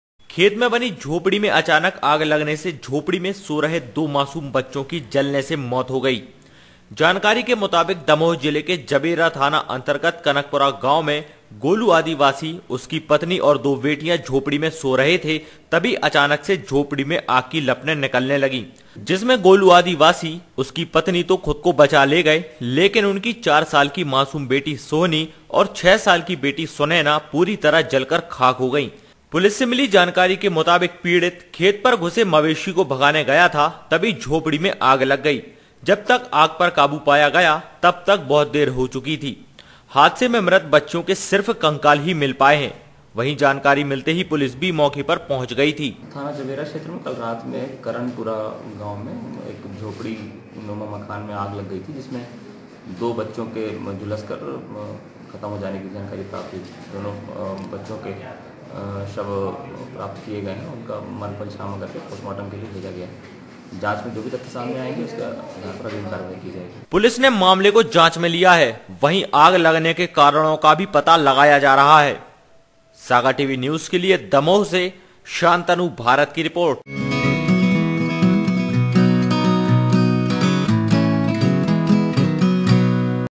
झोपड़ी में लगी आग में दो मासूम बच्चियां ज़िंदा जलीं
खेत में बनी झोपड़ी में अचानक आग लगने से झोपड़ी में सो रहे दो मासूम बच्चों की जलने से मौत हो गयी। जानकारी के मुताबिक दमोह जिले के जबेरा थाना अंतर्गत कनकपुरा गाँव में गोलू आदिवासी उसकी पत्नी और दो बेटियां झोपड़ी में सो रहे थे तभी अचानक झोपड़ी से आग की लपटें निकलने लगी। आग जिसमे गोलू आदिवासी और उसकी पत्नी तो खुद को बचा ले गए लेकिन उनकी चार साल की मासूम बेटी सोहनी और छह साल की बेटी सुनैना पूरी तरह जलकर खाक हो गयीं। पुलिस से मिली जानकारी के मुताबिक गोलू आदिवासी खेत पर घुसे मवेशी को भगाने गया था। तभी झोपड़ी में आग लग गई। जब तक आग पर काबू पाया गया तब तक बहुत देर हो चुकी थी। हादसे में मृत बच्छियों के सिर्फ कंकाल ही मिल पाए हैं। जानकारी मिलते ही पुलिस मौके पर पहुँच गयी थी पुलिस ने मामले को जांच में  लिया है। वहीं आग के कारणों का भी पता लगाया जा रहा है।